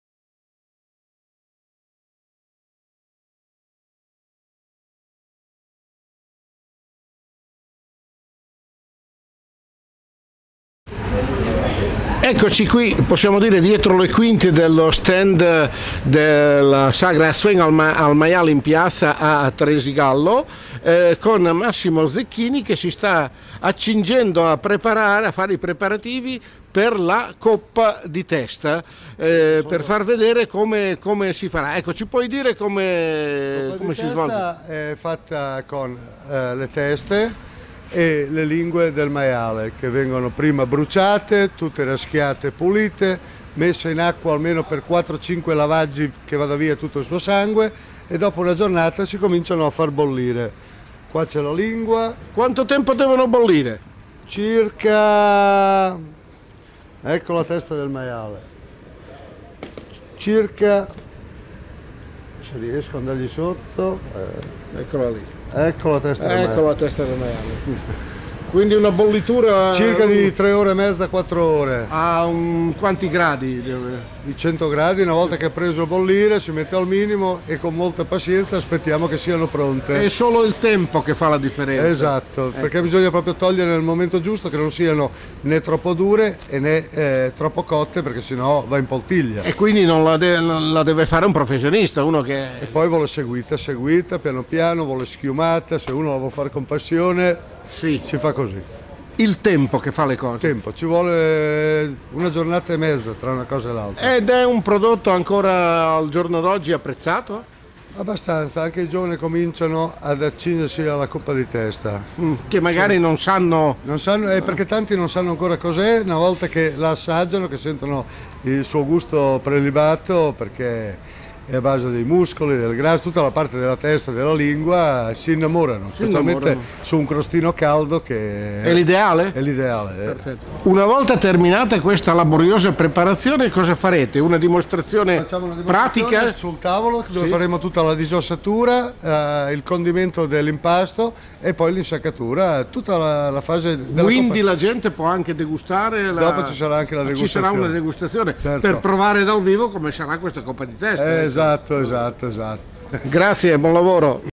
"A zzuen al mail ...in piaza! - Massimo Zicchini
Maurizio Barbieri intervista uno dei norcini della manifestazione, Massimo Zicchini che sta preparando la 'coppa di testa'. Parte tecnica, Giovanni Tavassi.